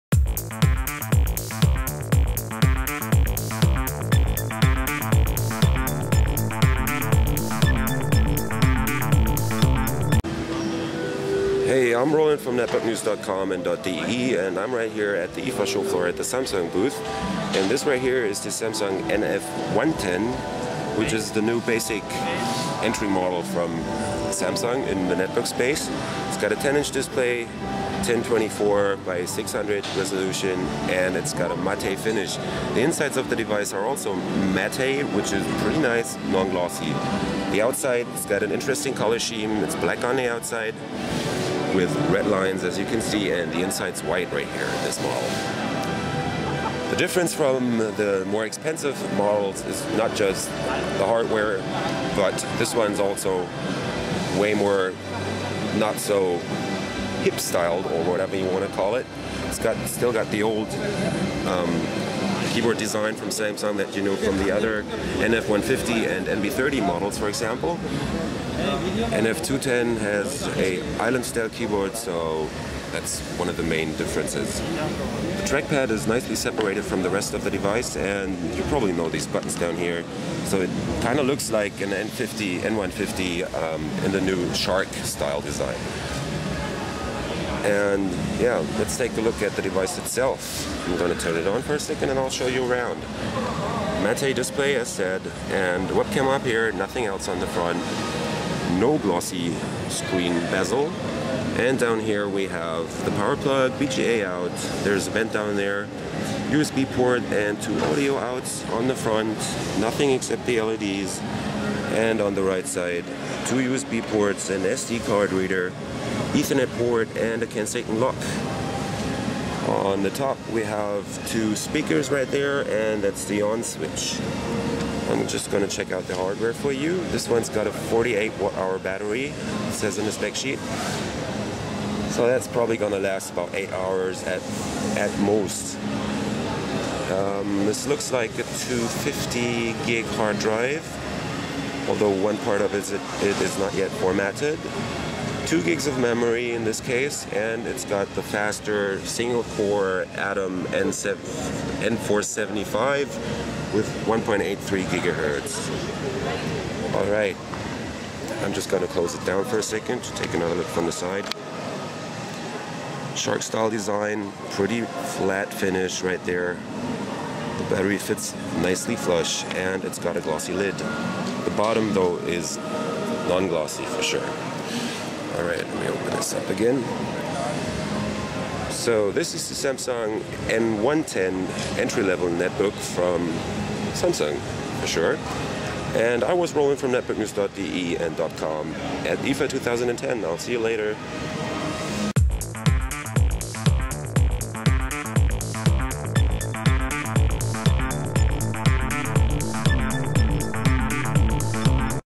Samsung NF110 10,1 Zoll Netbook mit Atom N475 CPU im Hands-on auf der IFA 2010
- Das Samsung NF110 ist das neue Einstiegsmodell der Koreaner und mit einem Intel Atom N475 ausgestattet.